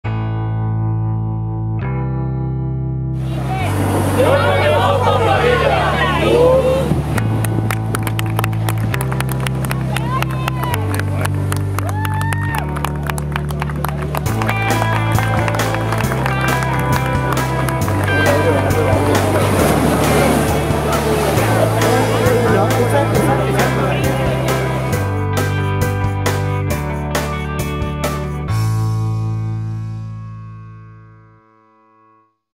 Los nadadores de la primera etapa de CabodeGataNijar
¿Que es mojateporlvida?
Nervios, ilusión, esperanza, solidaridad, compañerismo. la magia de un grupo de nadadores que disfrutan haciendo lo que más les gusta para poder ayudar a esos pequeños luchadores.

Imagenes resumen, de una primera etapa,...hoy alegría, agradecimiento y ganas de más,... mañana ya será otra historia...GRACIAS!!!